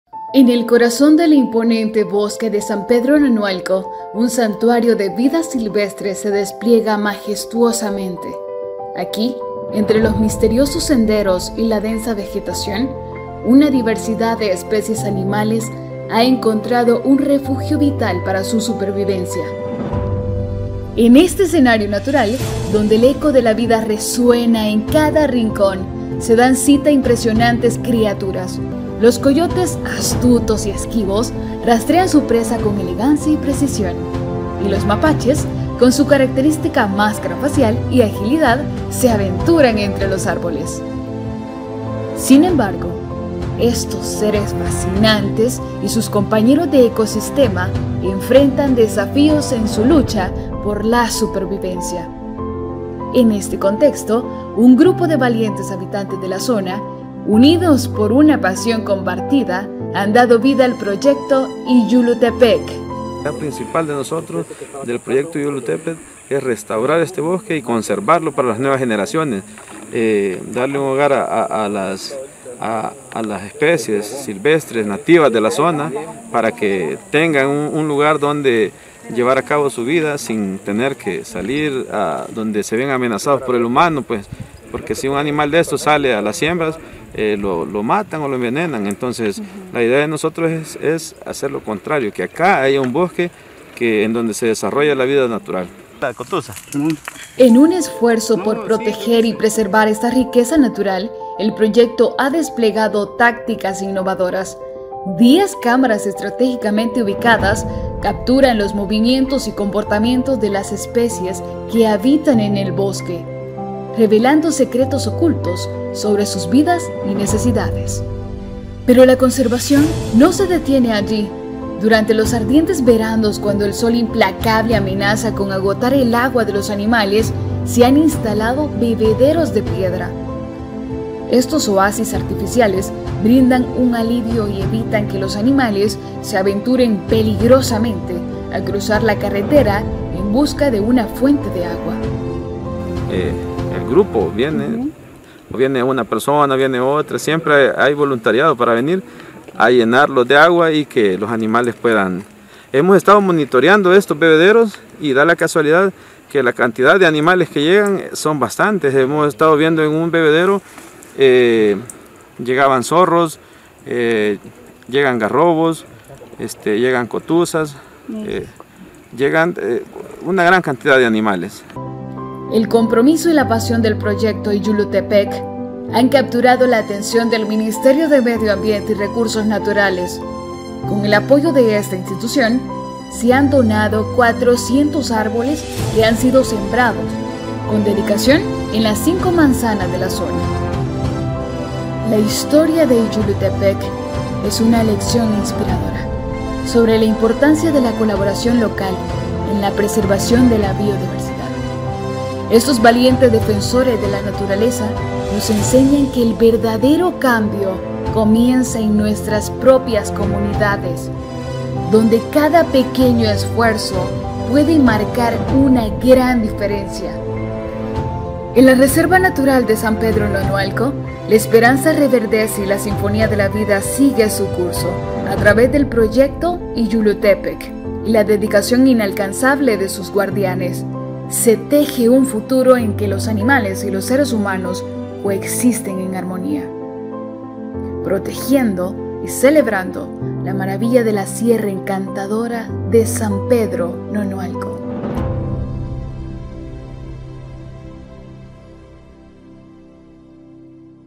Descubren 10 especies de animales exóticos en San Pedro Nonualco, La Paz
En el cerro Nahuistepeque, municipio de San Pedro Nonualco, se han instalado 10 cámaras trampa con las que se han ubicado a diferentes especies.